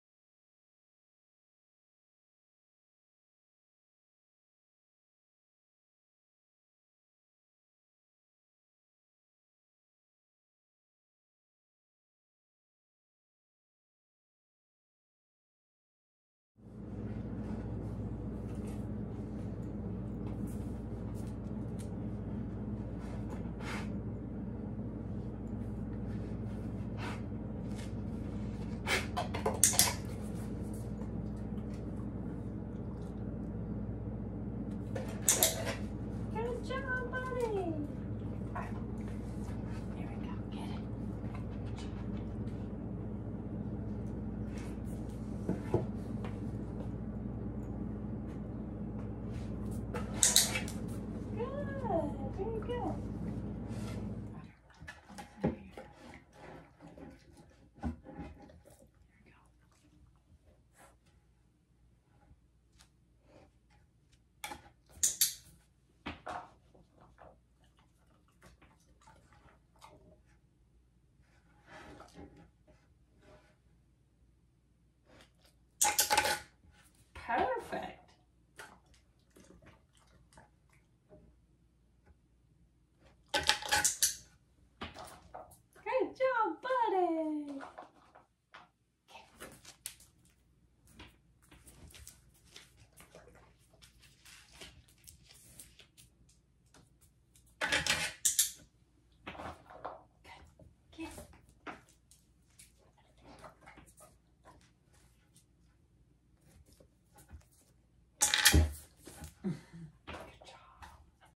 Cooperative Care: Canine Scratchboard Training
This video shows a brief demonstration of using clicker training to teach a dog to use a scratchboard with his front paws, which helps keep those nails to a desirable length. This is an overview of the process, intended to show that this can be a useful way to keep your dog's nails in good shape, while avoiding the fear, avoidance, and struggle that often accompanies traditional nail trimming using clippers. The dog in this video has had some previous paw targeting training, but this is only the second session with a scratchboard. The clicker is not easily visible, but is in my left hand, and I click it when he scratches the board, but if your dog is brand new to this, click anytime your dog moves a front paw. After clicking for paw movement for a while, begin clicking for paw movements toward the scratchboard. Over several short sessions (5 minutes max), shape the behavior into scratching the board laid flat on the floor, and then eventually to the board in a vertical position, for maximum friction on the nails. This process should always be fun and motivating for your dog! If it's not, reduce your criteria for rewards until you have success, then slowly build on that.